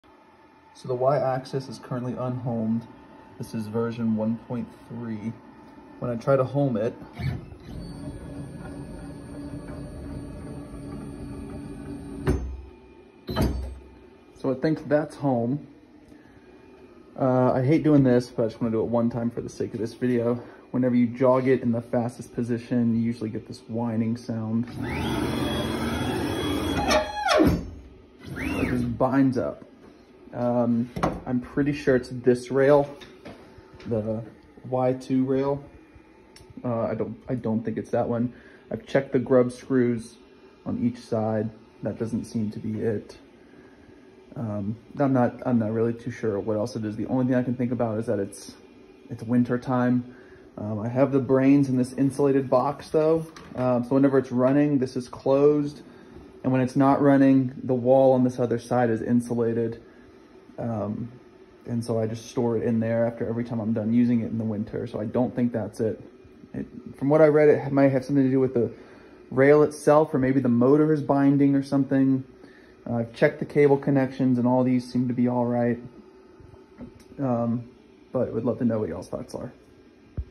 OneFinity journeyman not homing all the way and seizing when jogging on fastest setting issue
My onefinity x-50 journeyman was giving me two issues. The first was the Y axis, not homing all the way (like it would stop with still a few inches of rail showing and think that was home). The other issue was when I jog the machine at the fastest speed, there was some seizing going on and a nasty sounding whirr ensued. The motor was seizing up for some reason. Version 1.3.0

EDIT: I updated to the latest version (v1.6.1) and was able to fix the issue by lubricating the ball screw on the Y axis that was seizing up. I also re-lubricated the rails themselves and jogged the Y axis back-and-forth to distribute the lube. I used 3 in one multipurpose oil (black and silver bottle on amazon). Everything seems to be working as normal now with no seizing, and the machine is homing properly. My only concern is that the homing issue was occurring well before the seizing issue. So I don’t know if I actually solved the homing issue, but it’s working right now.